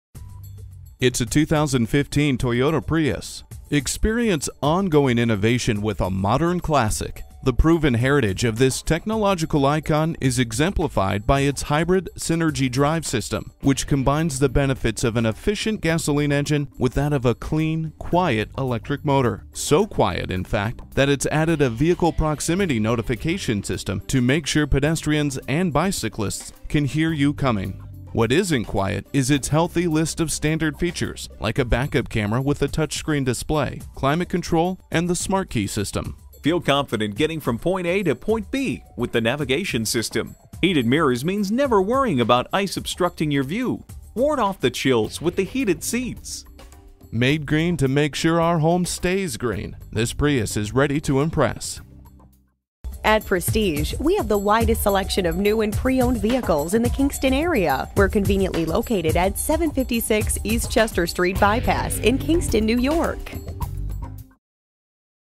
2015 Toyota Prius Plug-in Kingston NY Poughkeepsie, NY F3072999 - SOLD
Year: 2015
Make: Toyota
Model: Prius Plug-in
Trim: Base
Engine: 1.8L 4-Cylinder DOHC 16V VVT-i
Transmission: CVT
Interior: Dark Gray
Stock #: F3072999
VIN: JTDKN3DP7F3072999

Address:
756 East Chester St By-Pass